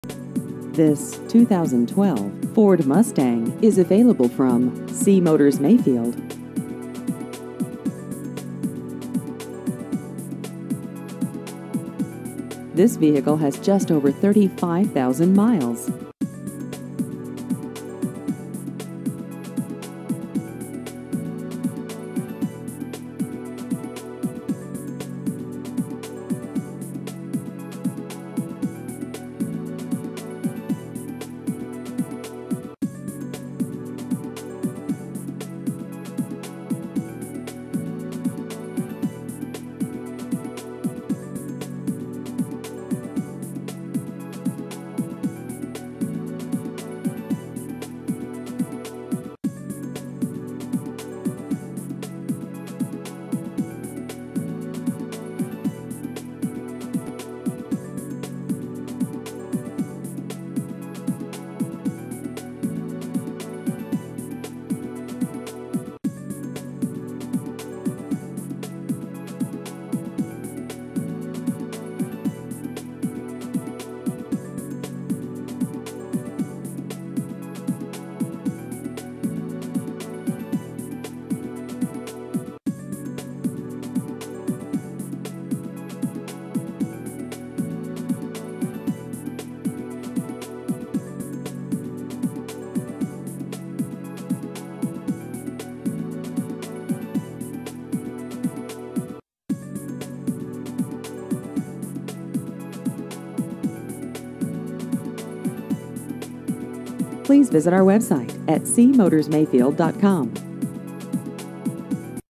2012 Ford Mustang Used Cars Mayfield KY
This 2012 Ford Mustang is available from Seay Motors - Mayfield.